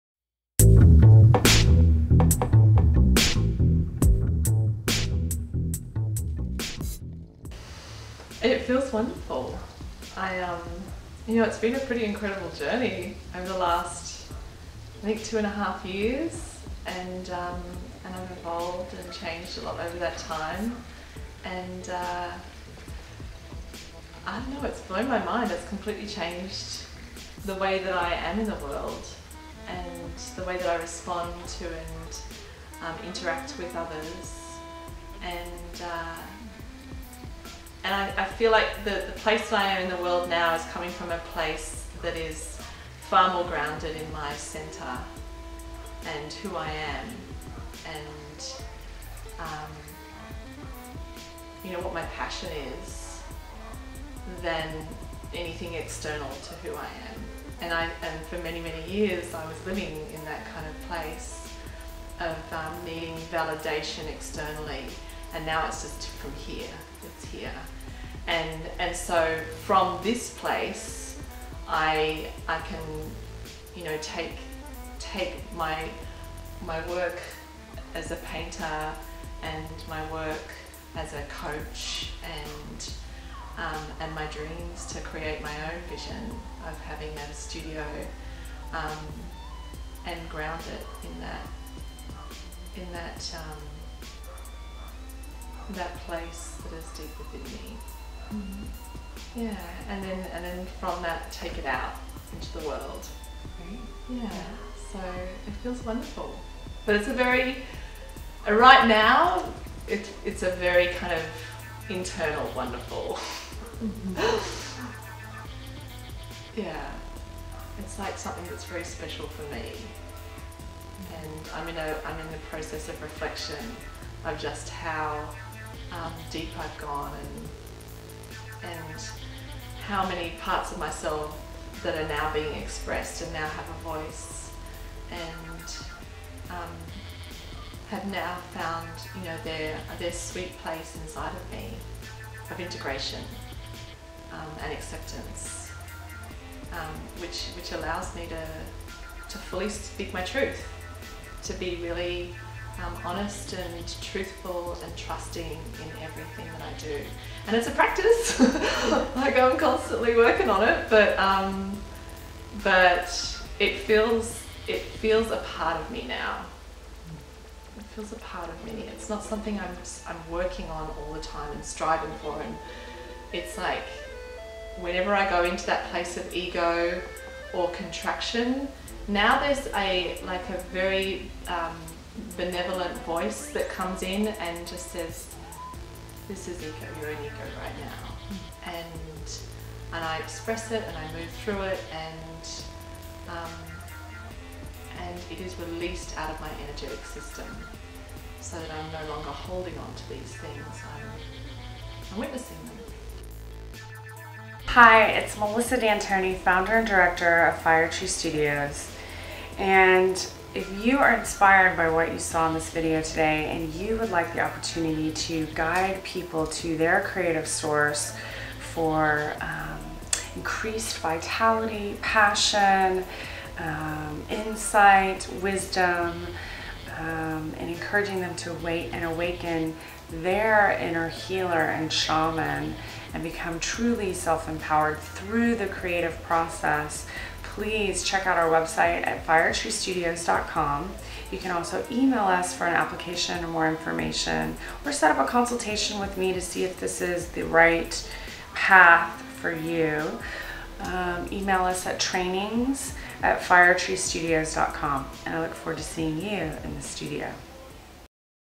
How Does it Feel to have Completed Your Training in The Fire Tree Method (tm)?
Rebecca talks about completing her Facilitator and Coaching Training in The Fire Tree Method (tm).